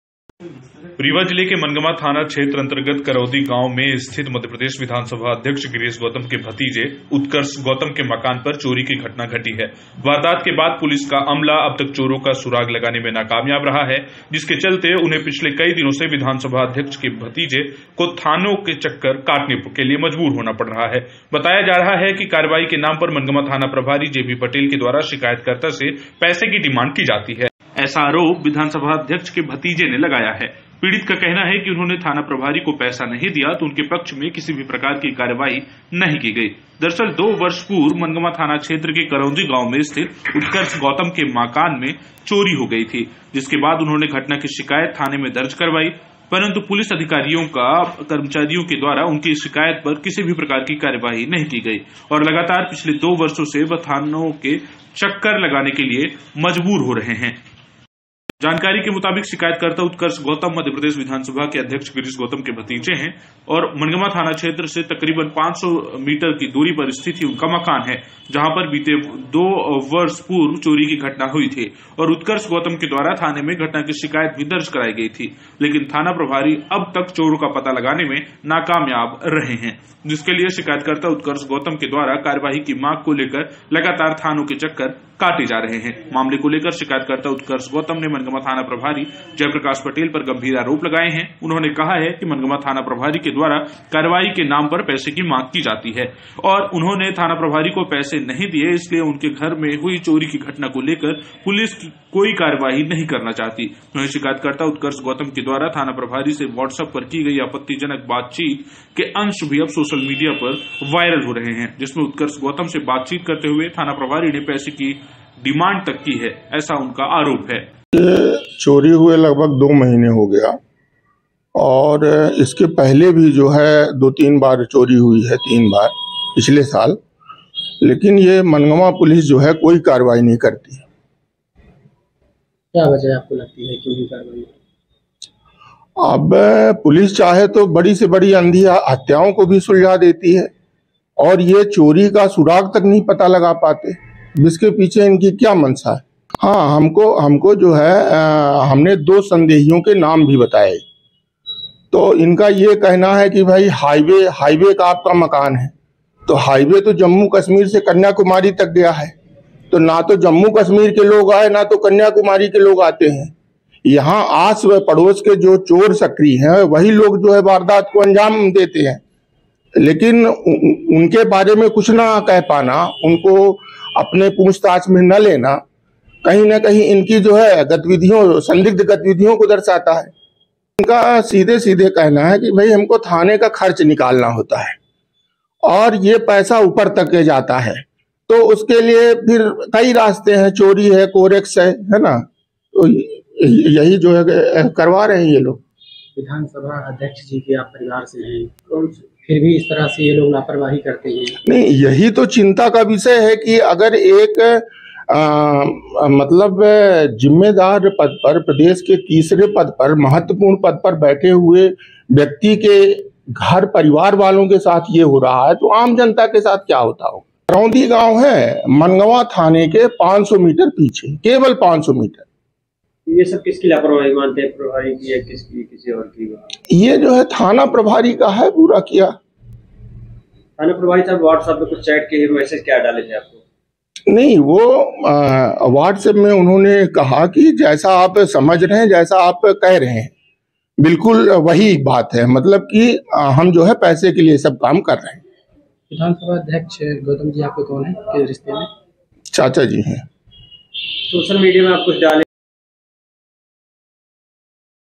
Rewa: न्याय के लिए भटक रहे MP विधानसभा अध्यक्ष के भतीजे, मनगवा थाना प्रभारी पर लगाए गंभीर आरोप
स्नसभा अध्यक्ष गिरीश गौतम के भतीजे उत्कर्ष गौतम के घर में एक सालों में कई बार चोरी की घटनाएं हो चुकी है, लेकिन उनका आरोप है की मनगवा थाना की पुलिस चोरी का सुराग लगाने में नाकामयाब है. विस अध्यक्ष के भतीजे का आरोप है की थाना प्रभारी पैसे की मांग करते है।,Assembly speaker|Crime News|mp news|MP Police|Rewa News|Theft Cases